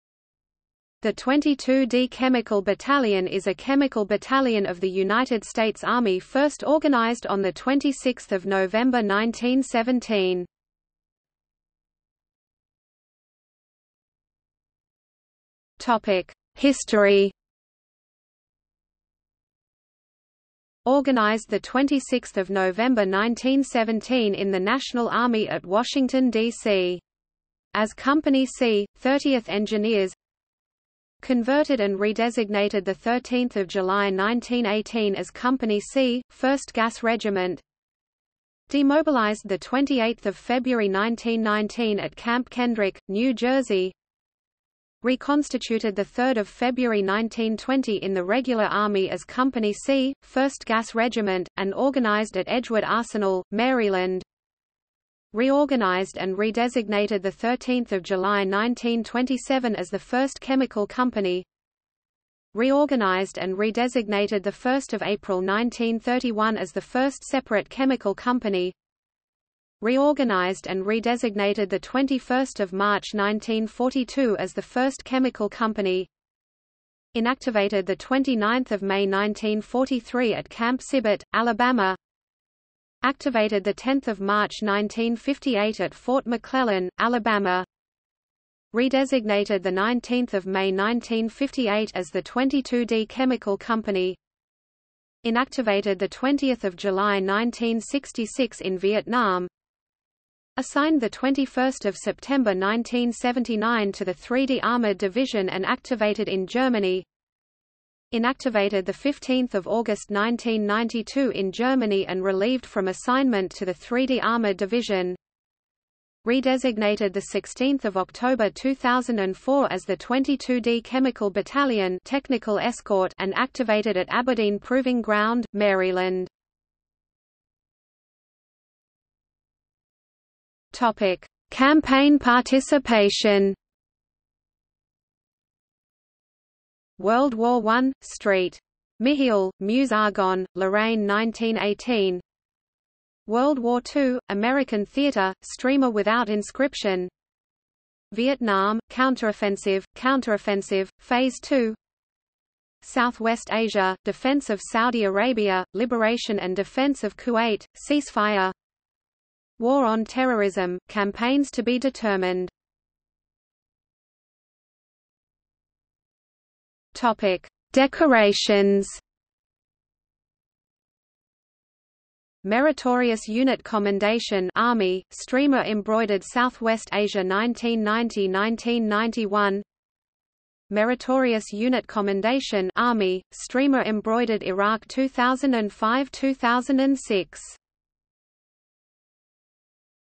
22nd Chemical Battalion | Wikipedia audio article
00:00:12 1 History
00:02:07 2 Campaign participation
00:02:43 3 Decorations

- Socrates

SUMMARY
The 22d Chemical Battalion is a chemical battalion of the United States Army first organized on 26 November 1917.